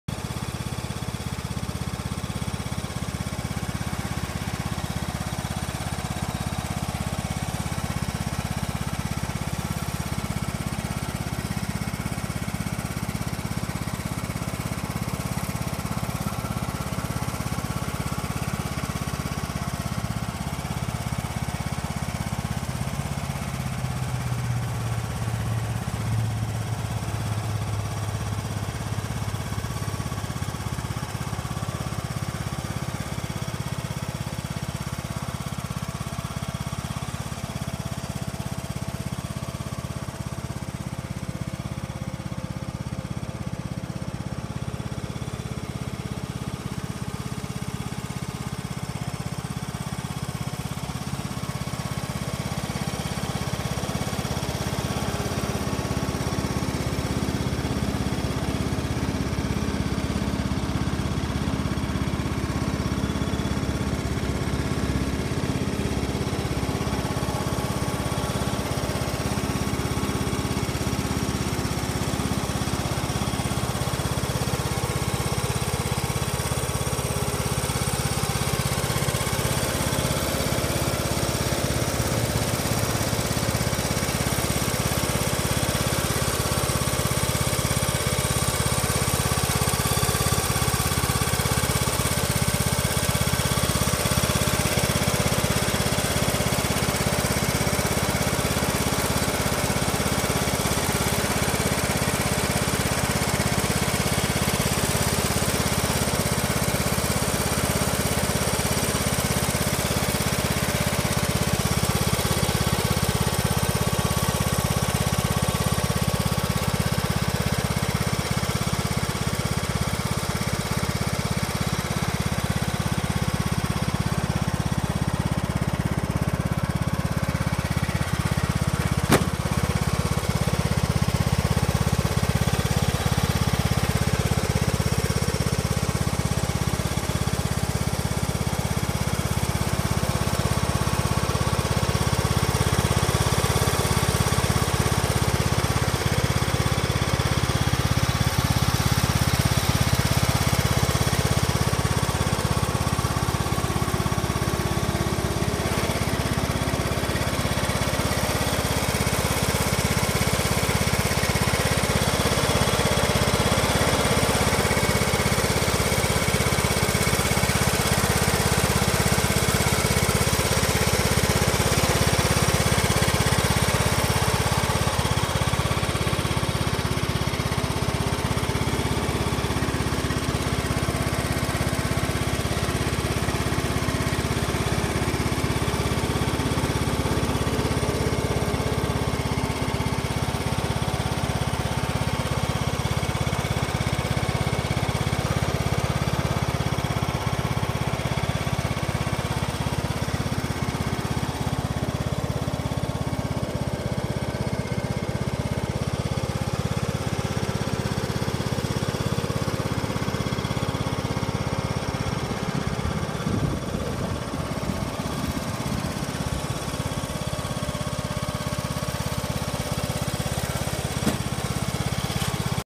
John Deere 100 Series Riding Lawn Mower
John Deere 100 Series Riding Lawn Mower (LA 125) , 2- Bag Bagger for 42-Inch and 48-Inch Mower Decks

This Lawn Tractor is currently available for sale by Michael F. Dilliard Auction Co, LLC For more info go